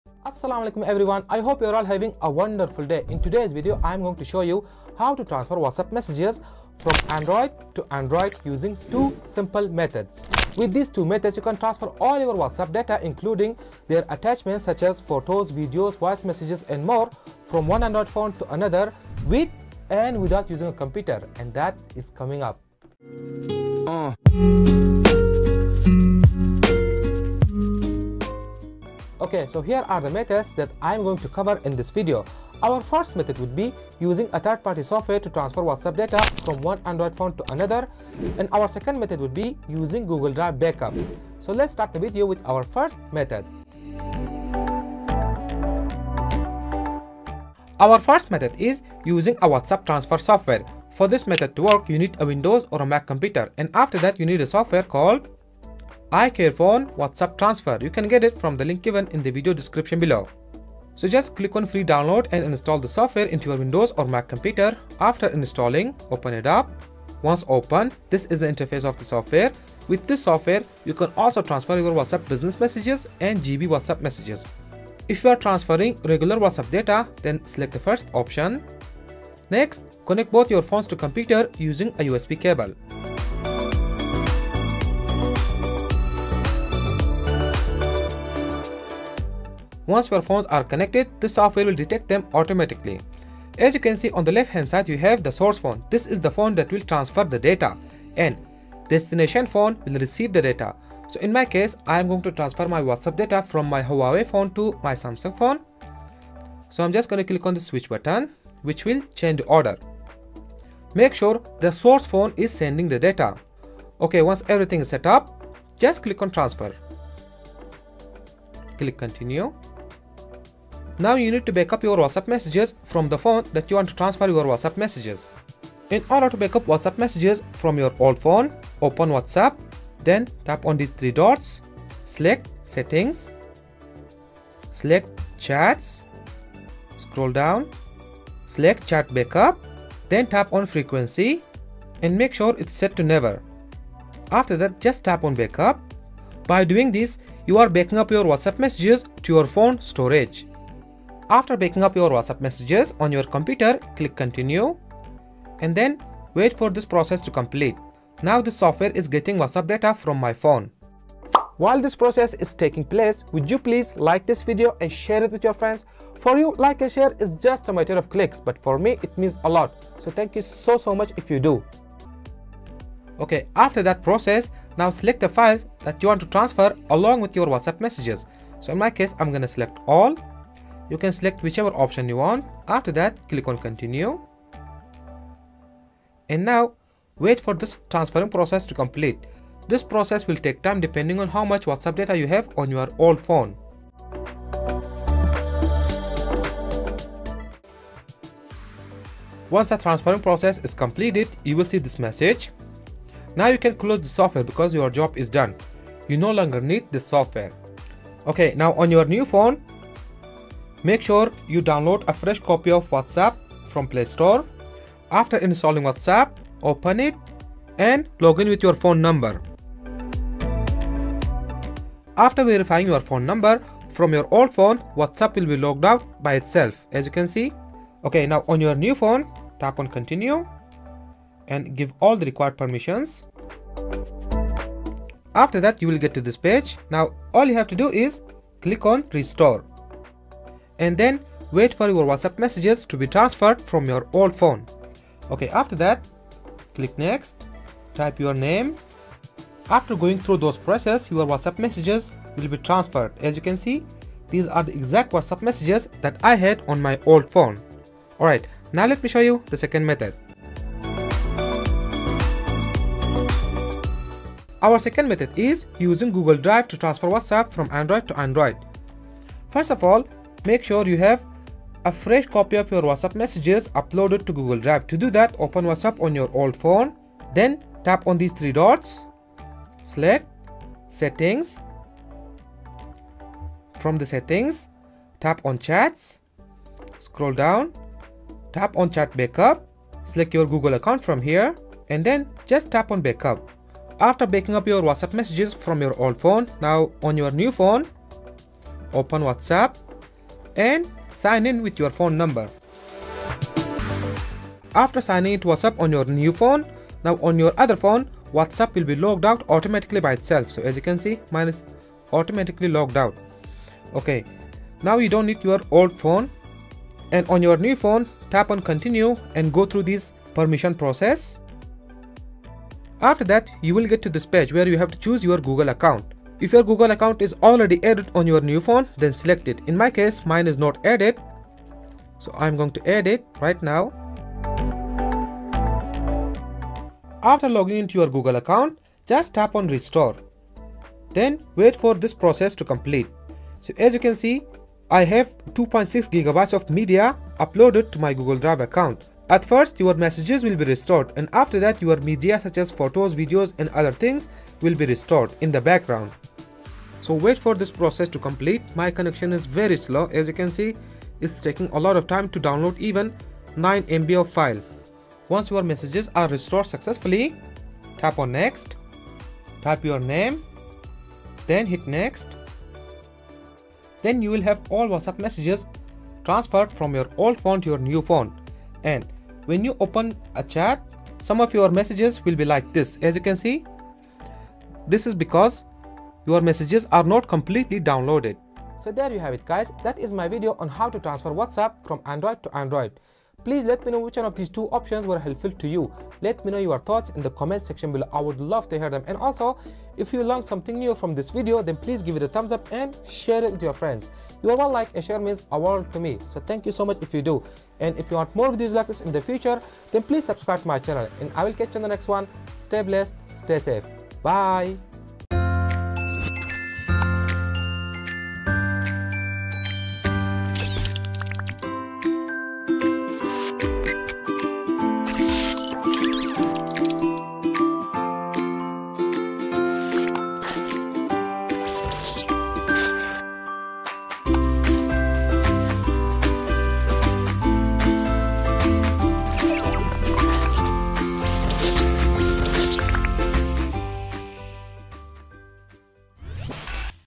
[2 Ways] How to Transfer Whatsapp DATA from Android to Android 2024
Learn How to Transfer Whatsapp DATA from Android to Android Using 2 Easy Methods.

► Method 1: Using Whatsapp Transfer Software
► Method 2: Using Google Drive

In this video, I’ll show you how to transfer WhatsApp data from Android to Android with ease! If you’re switching to a new phone and wondering how to transfer WhatsApp data to your new device, you’re in the right place. I’ll cover two different methods for WhatsApp transfer to new phone, so you can choose the one that works best for you. Whether you want to transfer WhatsApp messages from Android to Android or restore WhatsApp backup from Google Drive, I’ve got you covered. Plus, I’ll even share how to transfer WhatsApp messages to new phone without backup. Don’t miss out on these tips for a smooth WhatsApp data transfer!

⏰Timestamps:
0:00 Plz Like And Share
0:44 First Method.
04:20 Second Method.
06:28 Outro